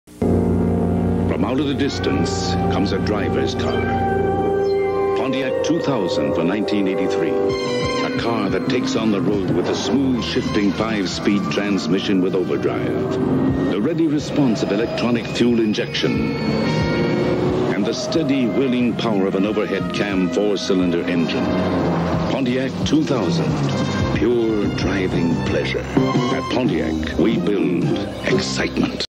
1983 Pontiac 2000 commercial
U.S. market GM commercial for the Pontiac 2000 hatchback.